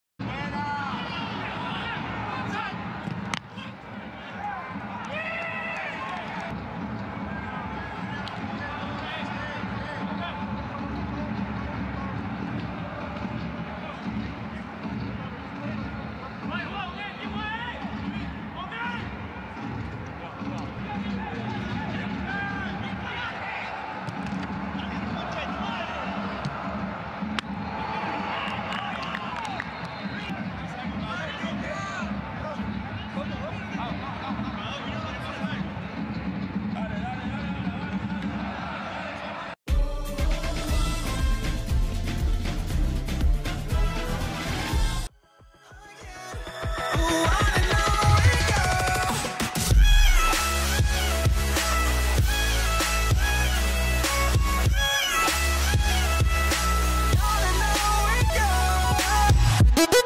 INCRÍVEL! CHAPECÓ FAZ DEFESAS MILAGROSAS NOVAMENTE, CONTRA LDU!
GABRIEL CHAPECÓ FEZ MILAGRES NOVAMENTE, ONTEM CONTRA A LDU!